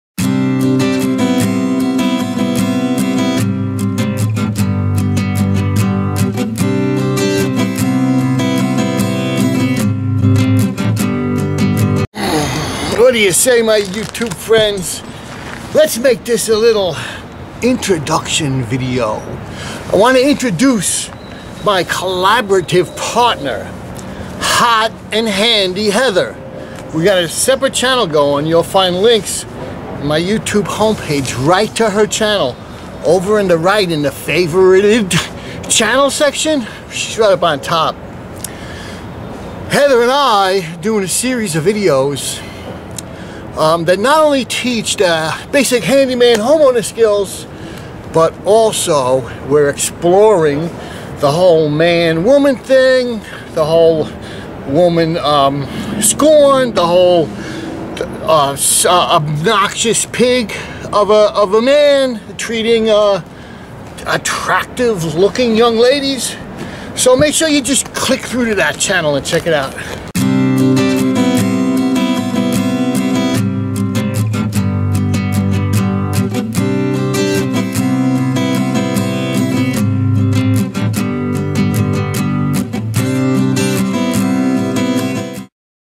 HotnHandy Heather collab DinoPinch
YouTube's own HotnHandy Heather in collab THZ intro by Dino Pinch
HotnHandy Heather different tricks to get that broken key out of any lock
how anyone can remove a broken key from any lock with common  household tools. doors, cars, trucks, boats, helicopters, snowmobiles, padlocks,  heavy machinery. simple demo video from
quick and informative broken key removal vid from the handyman zone. watch and learn how to get the broken key stub out of any lock with common household tools, saving the locksmiths minimum $300 service call charge

using a cheap lock for visual plaY , although the same methods will work for any type or manufacture of lock. home, auto, truck, boat, aircraft, heavy machinery , safes. YouTube Channel